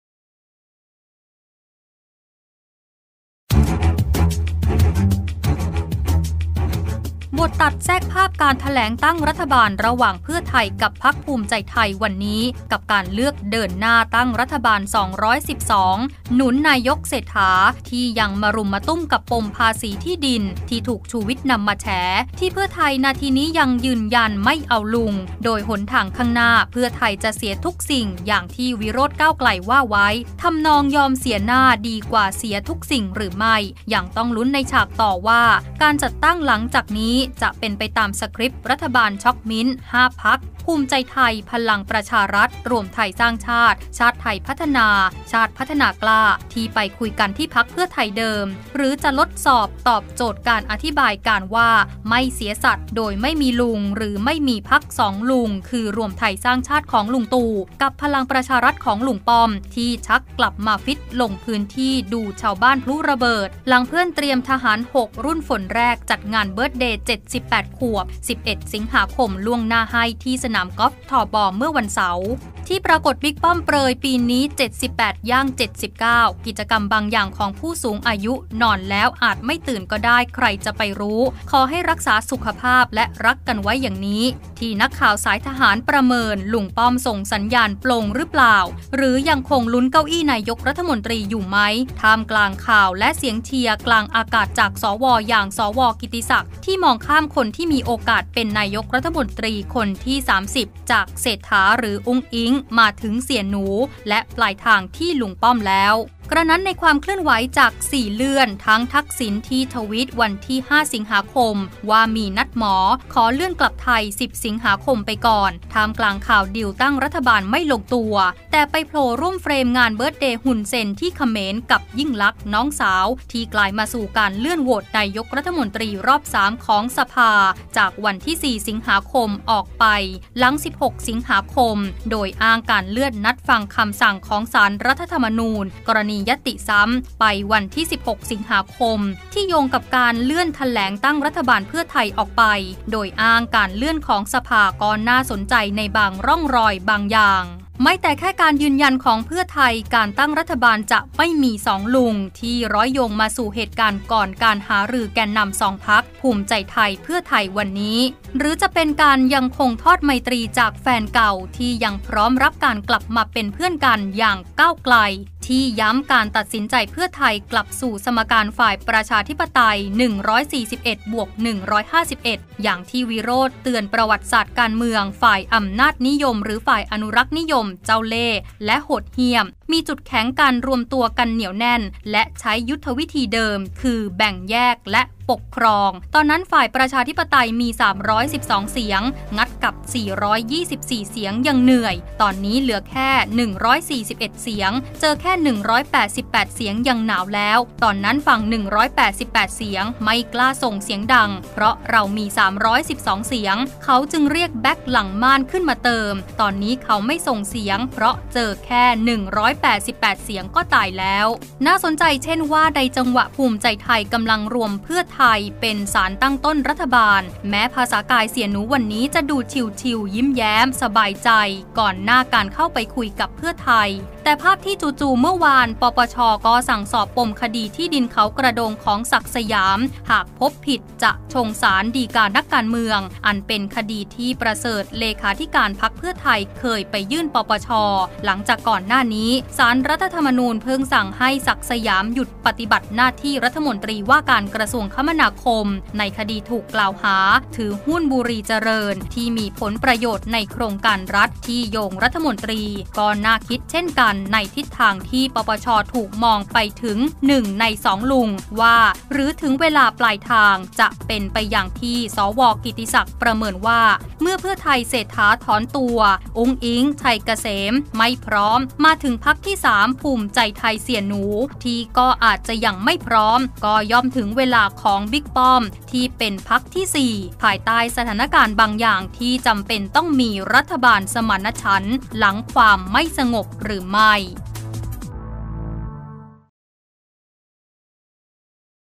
พท.+ภท. รัฐบาล212 ไม่มีลุง? : 07-08-66 | iNN Top Story
บทตัดแทรกภาพการแถลงตั้งรัฐบาลระหว่างเพื่อไทย(พท.) กับ พรรคภูมิใจไทย(ภท.) วันนี้(7ส.ค.)กับการเลือก “เดินหน้า”ตั้งรัฐบาล212 หนุน “นายกฯเศรษฐา”